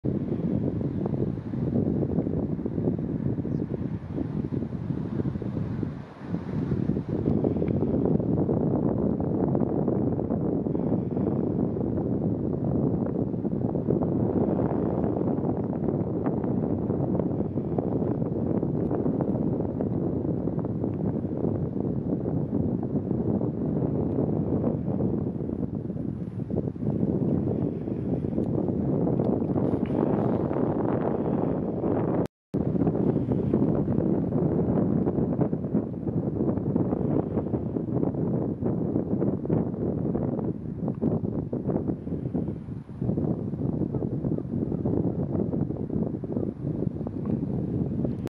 ハシボソガラスのクルミ落とし
風があったので土手のほうに流されていたけど、クルミを見失うことなく防草シートのところに持ってきて食べていました。